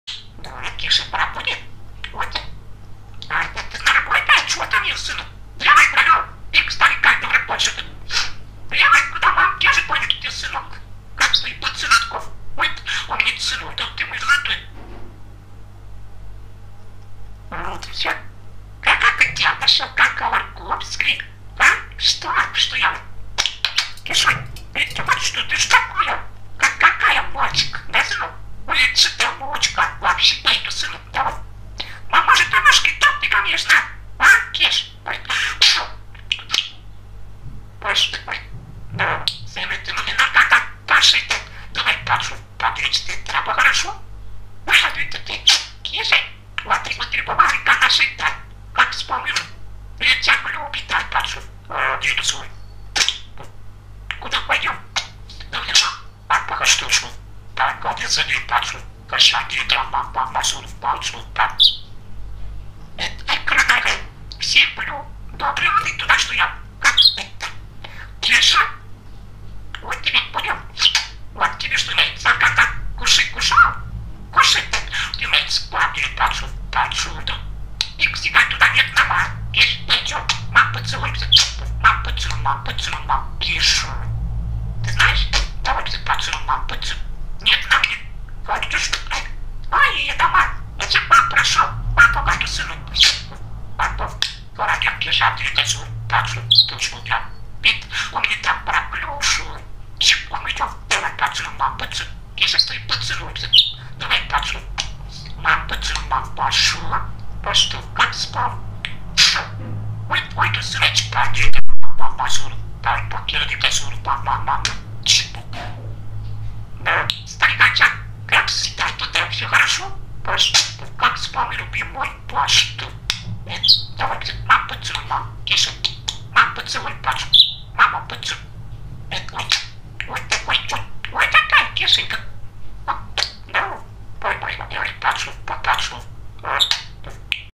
Болтливый и смешной попугайчик Кеша 😀
Говорящий попугайчик Кеша очень болтливый и смешной мальчик.
Всегда рады новым друзьям.